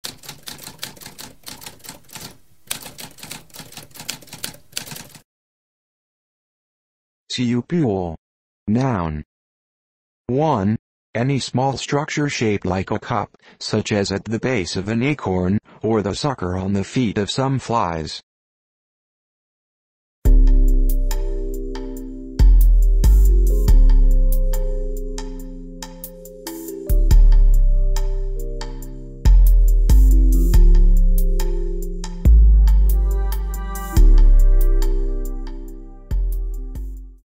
What does cupule mean?
A spoken definition of cupule.

Intro Sound:
Typewriter - Tamskp